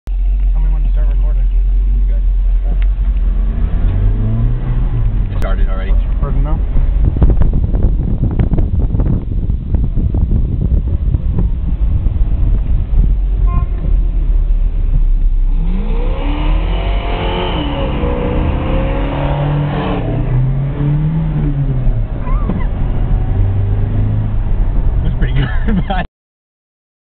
torino pass 2.MOV
1968 Ford Torino Passing Closed Road